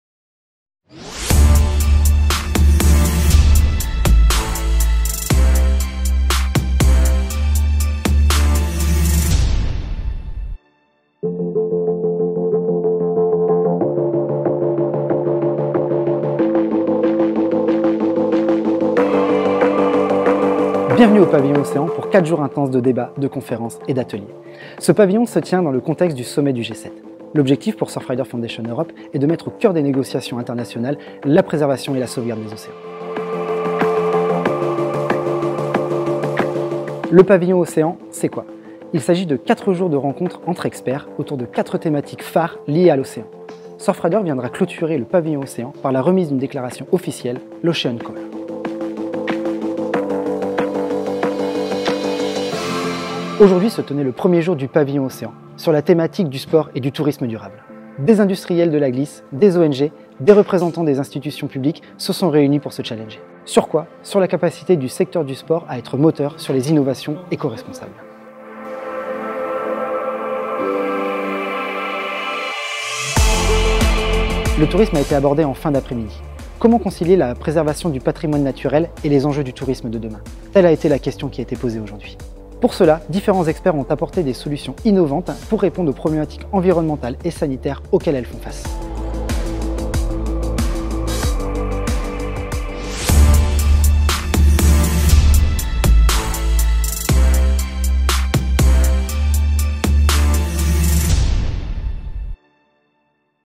Ocean Pavilion Day 1 Recap: Sports & Sustainable Tourism
From August 20-23, 2019 Surfrider Europe organizes Ocean Pavilion, a 4-day conference to encourage a focus on ocean conservation during international negotiations ahead of the G7 Summit in Anglet and Biarritz, France.

The theme of Day 1 was Ocean, Sports & Sustainable Tourism. Watch the recap of the day's events!